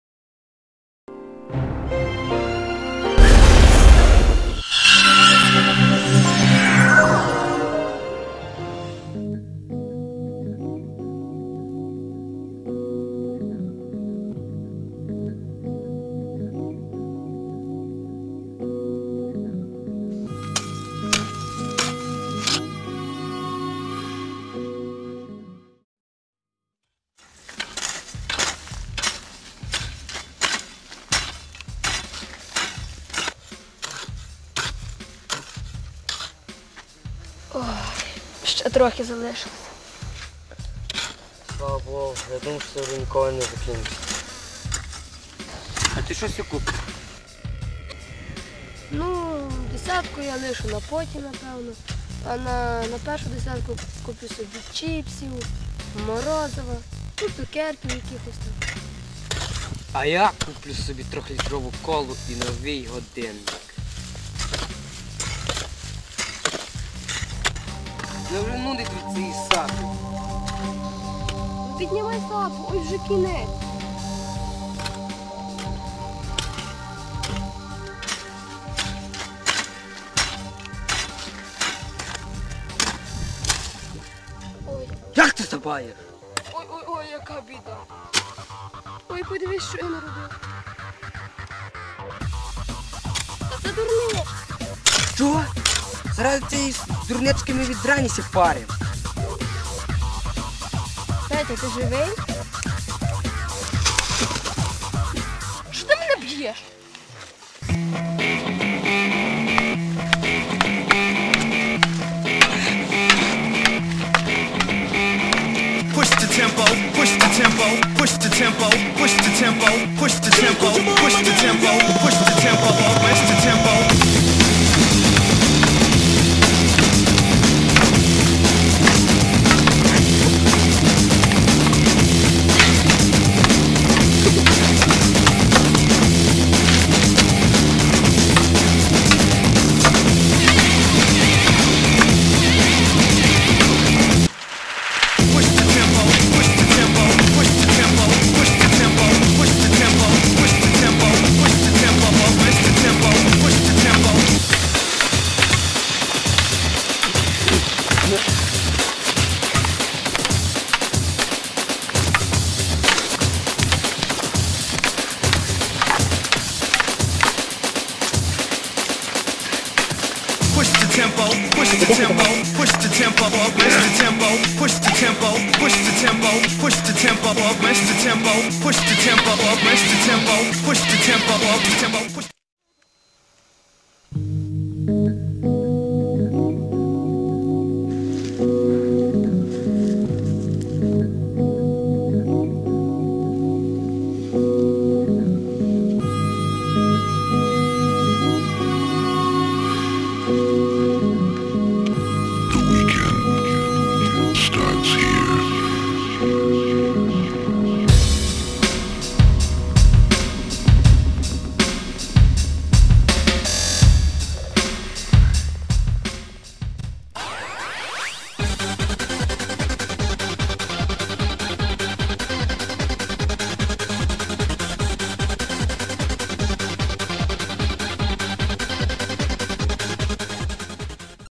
Одного разу у полі
Комедійний ролик, знятий в українському селі на полі. 2005 рік.